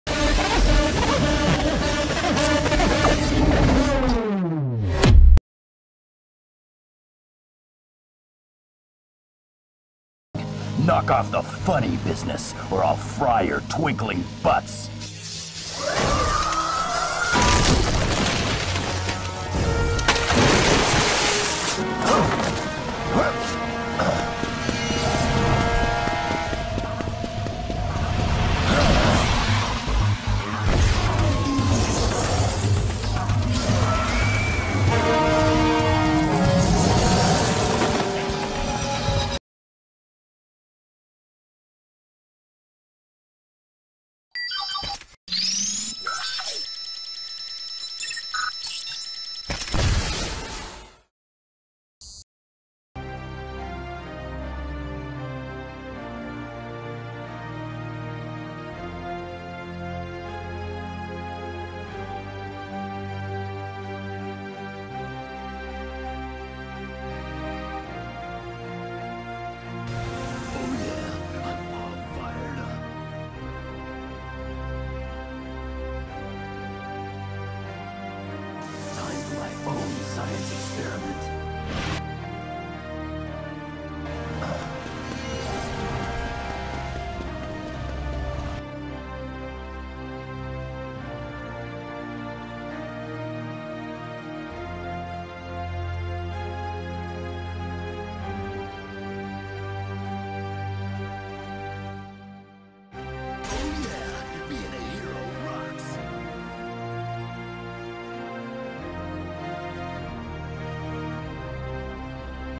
Ben 10 Heatblast running down a wall in real life.(Remake)
I recreated heatblast from the classic Ben10 series.